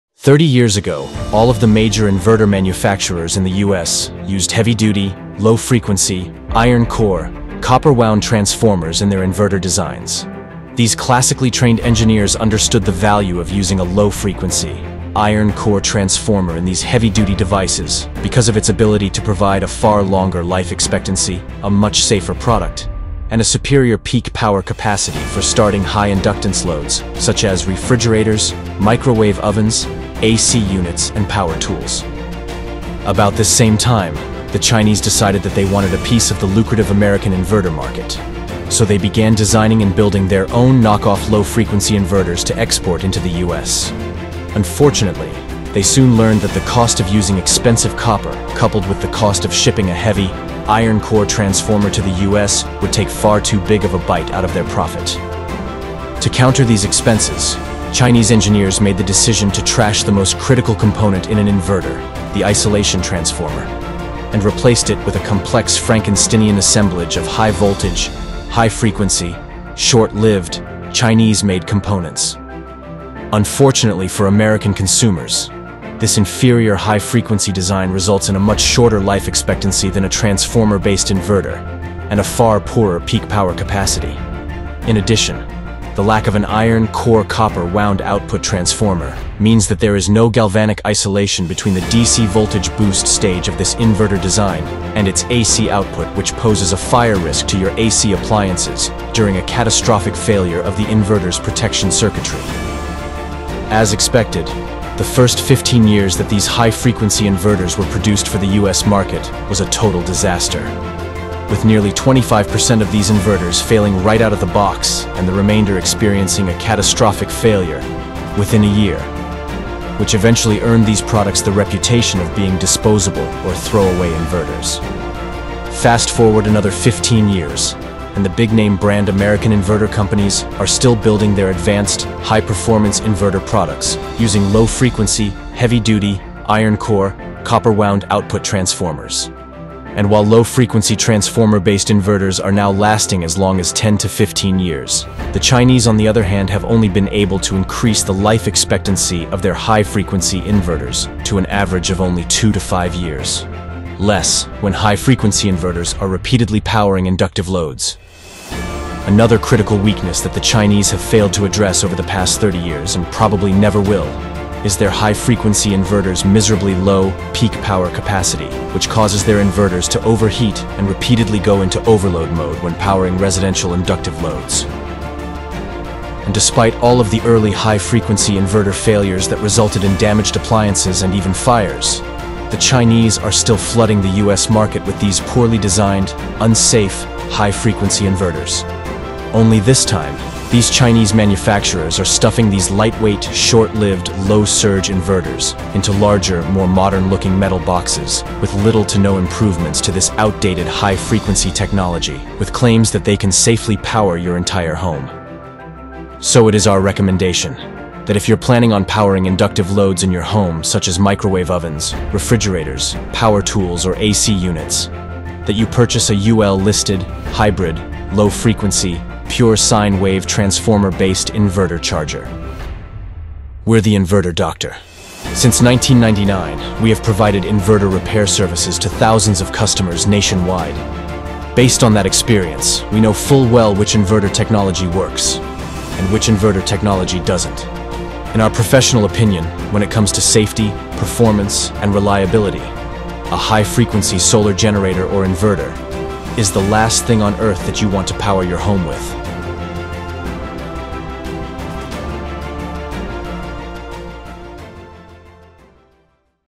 Low Frequency Inverter VS High Frequency Inverter
"6000xp is not UL9540 certified. YOU CANNOT LEGALLY install in the US along with solar panels."

For a more detailed explanation regarding the use of high frequency inverters for charging an EV, please watch Solar Inverter Catastrophic Failure:

The high frequency technology that's used in all larger high frequency inverters is basically the same tech that's used in the Ecoflow portable power stations.

Since the early 1990s, high frequency, transformerless inverters gained the reputation of being disposable inverters or (throwaway inverters) and were meant only for short lived, temporary installations. They were never designed nor meant for permanent installation to safely power a home’s appliances, at least, not for very long.

Even today, after more than 30 years of product development, You'd be lucky to get even two to three years of service from these Chinese made high frequency inverters, especially when powering high inductance loads like full sized refrigerators, large power tools, portable AC units or large microwave ovens. The reason for this is that all of these lightweight, high frequency inverters lack a very critical component. That critical component is an iron core, copper wound output transformer. Without an output transformer to act as a buffer to absorb the electrical surges provided by the “Flywheel Effect” inherent in the physical amount of a transformer’s iron, these surges and damaging reverse voltage spikes must be handled directly by the inverter’s MOSFET transistors, essentially shortening their life.

You might be able to start and run some high surge loads when these high frequency inverters are new, but every time you fire up one of those inductive loads, you're shortening the life of the MOSFETs in these inverters. To make things even worse, lightweight, high frequency inverters run much hotter and their internal components are far more stressed due to their high switching speeds and use of high voltage components, These two factors makes the low cost, off spec, Chinese made components such as capacitors, MOSFETs, diodes, resistors and ICs that are used in these inverters, far more prone to early failure. Simply put, high frequency inverters are better suited for like duty, electronic devices, and low frequency inverter are better suited for heavy duty, high peak power appliances.

Another major consideration with many of these lightweight, high frequency, transformerless inverters is safety. First of all, high frequency inverter don’t have Isolation between the Neutral and Line which increases the risk of a shock hazard. And second, during a catastrophic of the failure of the inverter’s circuitry, all it would take is for the AC output monitoring circuitry to fail and one of the MOSFETs in the inverter's H-Bridge circuit to short to ground, and these high frequency inverters can send dangerous, high voltage, high amperage DC current straight to your connected AC loads which will not only damage most AC appliances but can also set those AC appliances on fire.

After the warranty runs out, you're not going to be able to ship these units back to the manufacturer in China and have them repair them. And at a typical shop rate of $125 per hour here in the US, with no schematic and limited parts availability, it won't make economic sense to even attempt to repair one of these units. Once the FETs have fried and have burned a good portion of the inverter's PCB, (And trust me, sooner rather than later, they will fry.) they basically become a brick.

A much better choice would be to invest in a UL-1741 listed hybrid inverter and a bank of Lithium Iron Phosphate batteries and an inverter that uses low frequency, transformer based technology in its design. Low frequency inverters can handle high surge loads for at least 3x (300%) their continuous rated capacity and they can do this repeatedly, without sustaining damage to their MOSFET transistors, for minutes, rather than the milliseconds that a high frequency inverter offers. That's why the big name brand inverter manufacturers like Schneider Electric, Outback Power, Sigineer Power, Magnum Energy and others, all use a low frequency topology in their design.